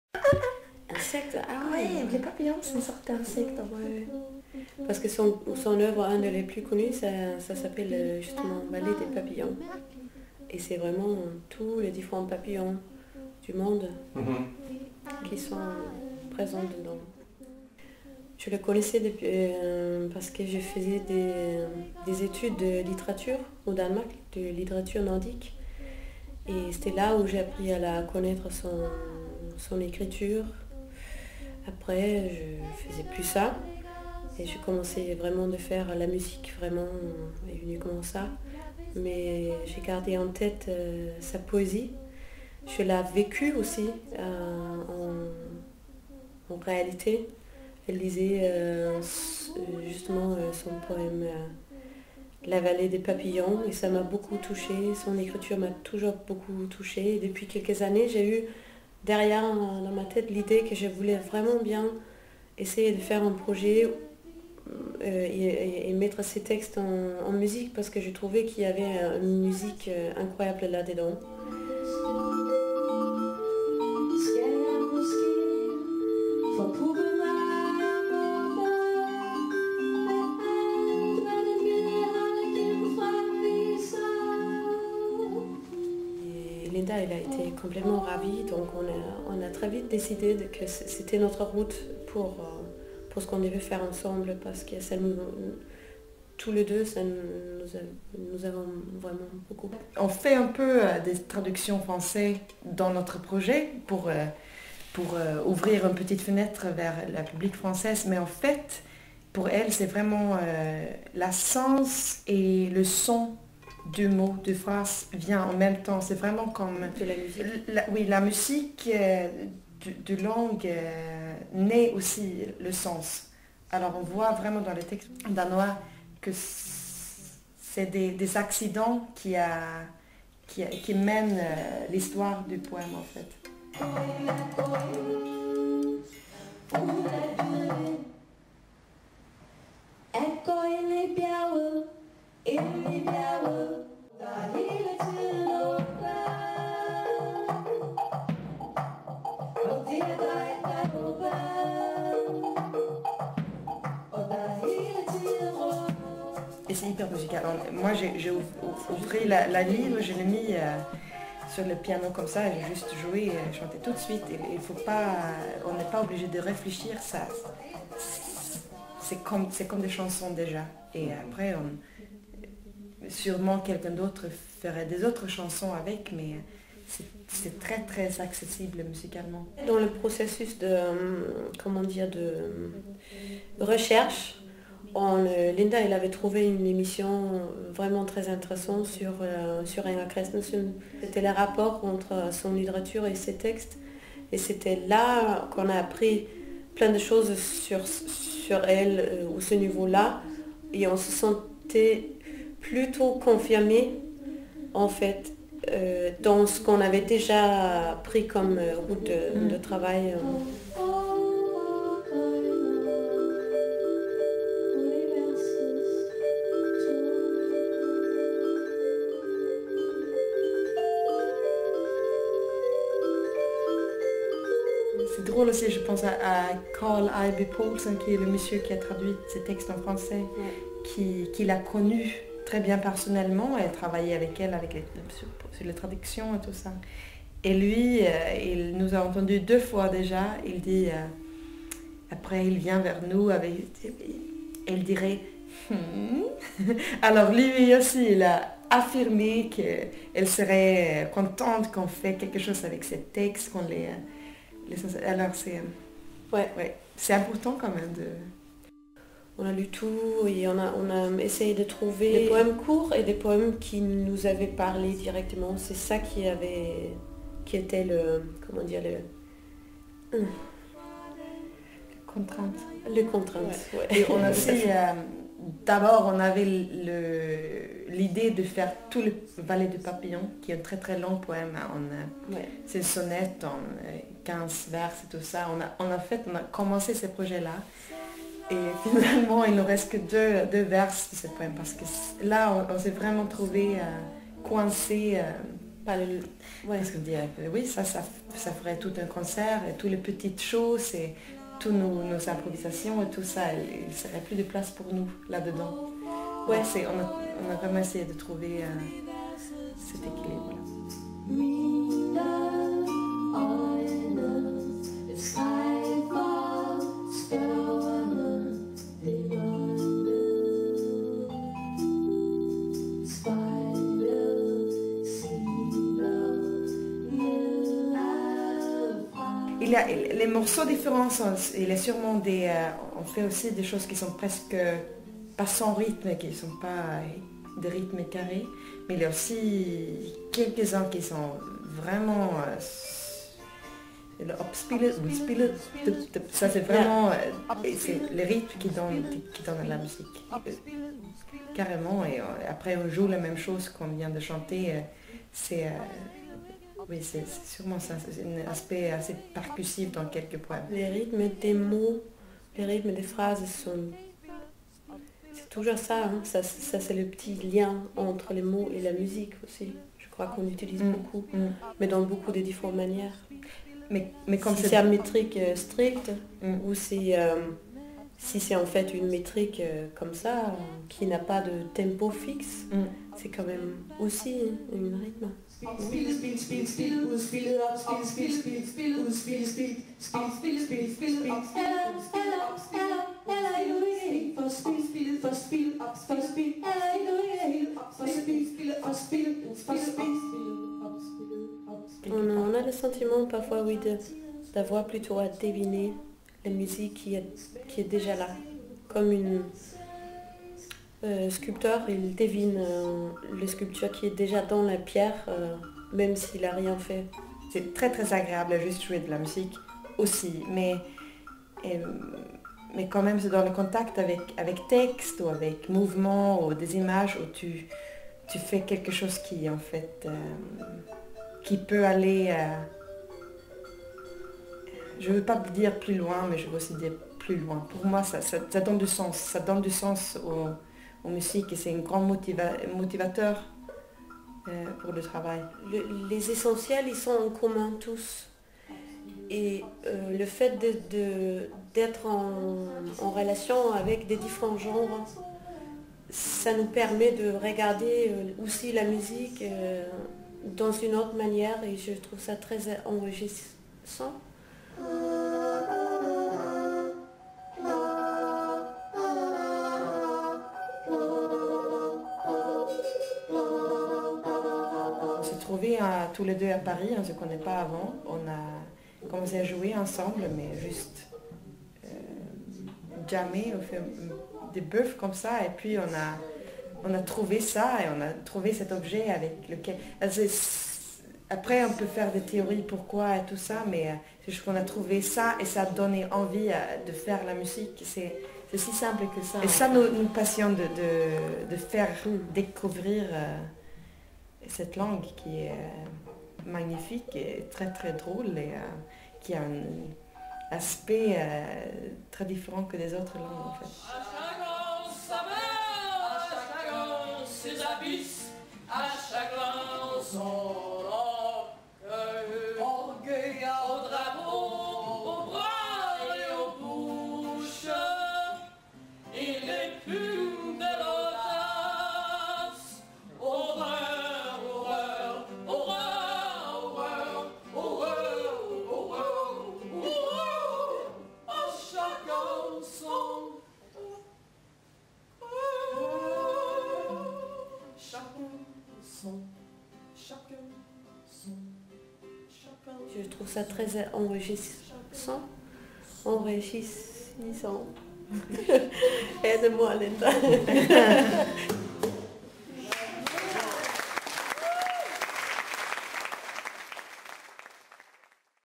INGER (#036) IN-SECT.tv
Derrière le projet Inger se cachent la percussionniste suédoise Linda Edsjö et la chanteuse danoise Birgitte Lyregaard. Ce duo a eu un véritable coup de foudre pour l'écriture très musicale et structurée de la poétesse, romancière et essayiste, Inger Christensen (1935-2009) qui est considérée comme la plus grande écrivaine expérimentale danoise de sa génération. Une écriture pleine d'humour, de rythme, de sagesse et marquée par "les forces de la nature". Un projet qui mêle compositions et improvisations aux textes de cet auteur, entre versions originales et traductions, faisant tomber les barrières entre musique des mots et celle des instruments donnant aux textes une saveur nouvelle. L'envie pour nous de rencontrer ses deux musiciennes scandinaves.

L'enregistrement de cette vidéo a eu lieu au printemps 2011, avant donc le décès en juillet de Karl Ejby Poulsen, Directeur de la Fondation Danoise pendant 25 ans et traducteur de Inger Christensen.